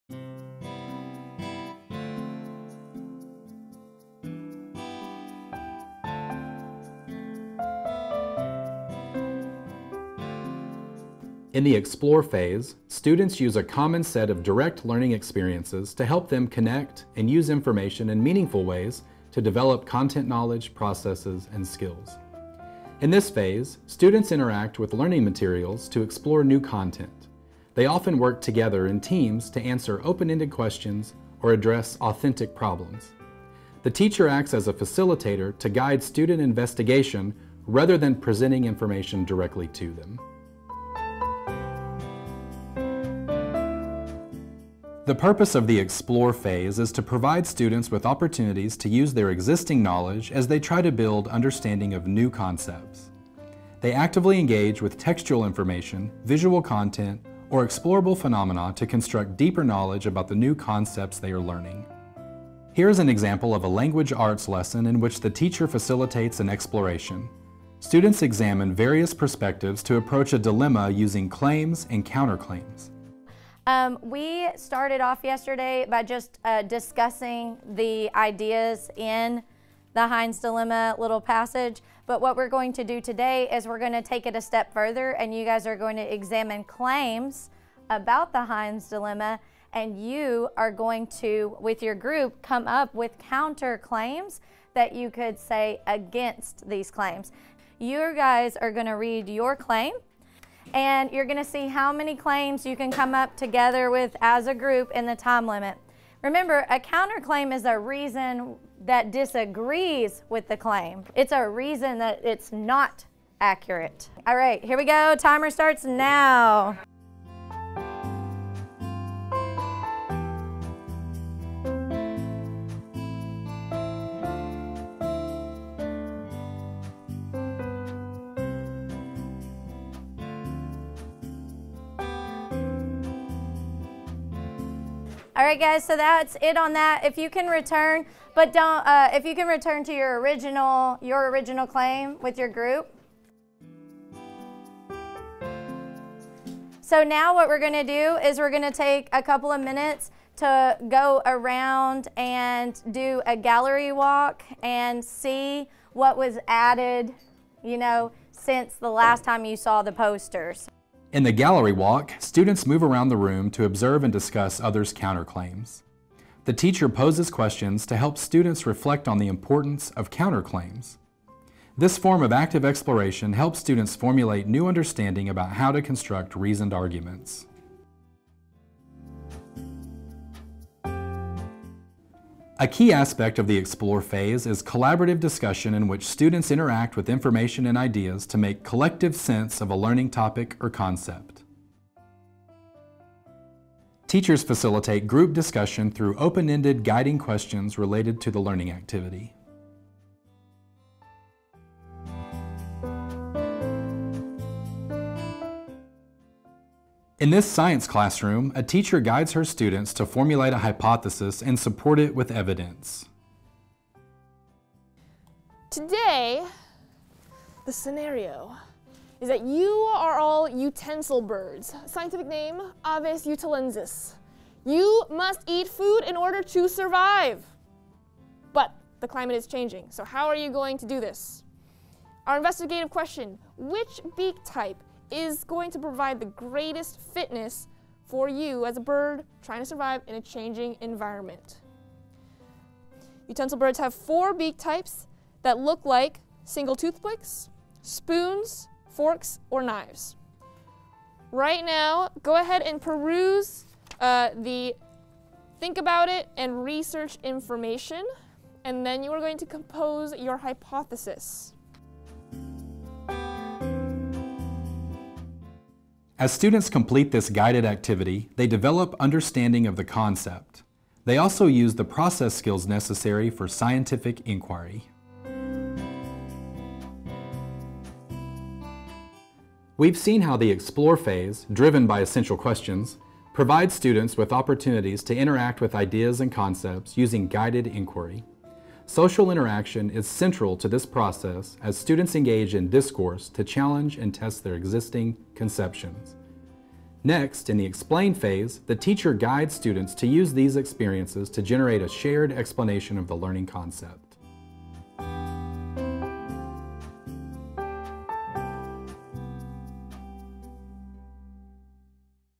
The 5E Instructional Model: Explore Phase
This video describes essential aspects of the Explore phase of the 5E instructional model. You will see classroom examples of teachers guiding students as they actively engage in the exploration of new content.